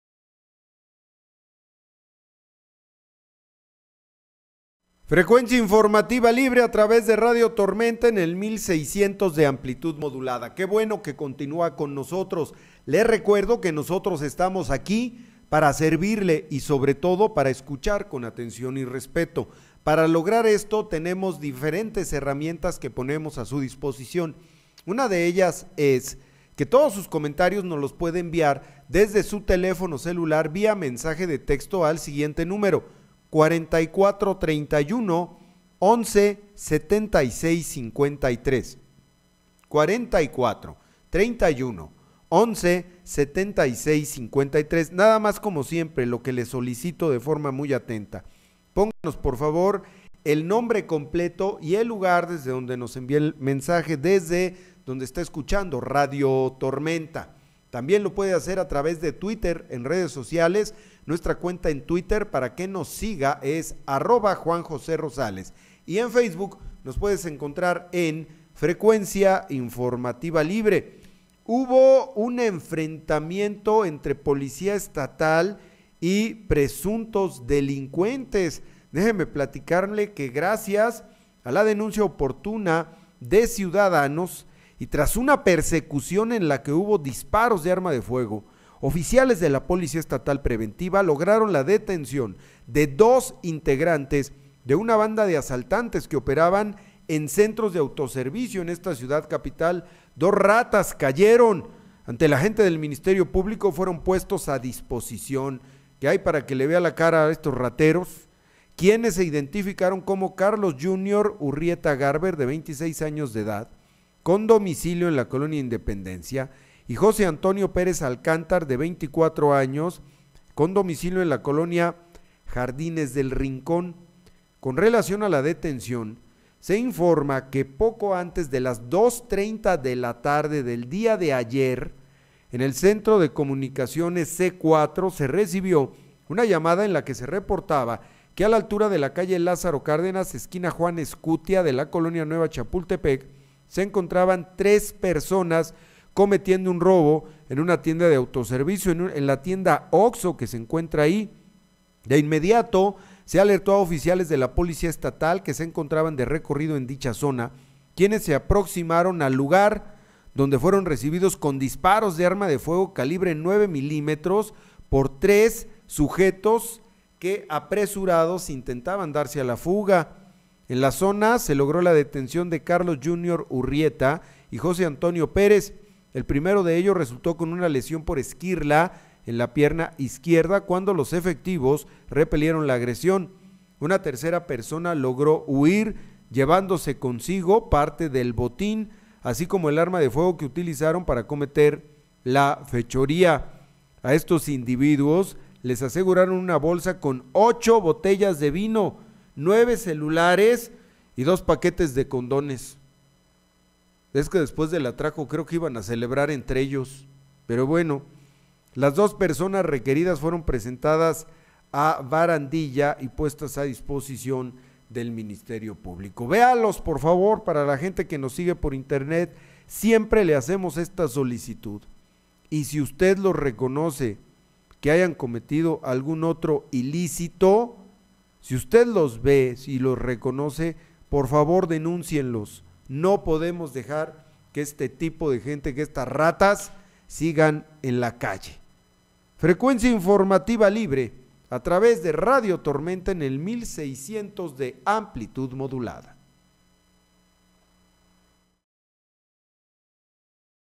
SE ENFRETAN A BALAZOS Y SON CAPTURADOS POR POLICIA ESTATAL
SE ENFRETAN A BALAZOS CON POLICIA ESTATAL - Captured Live on Ustream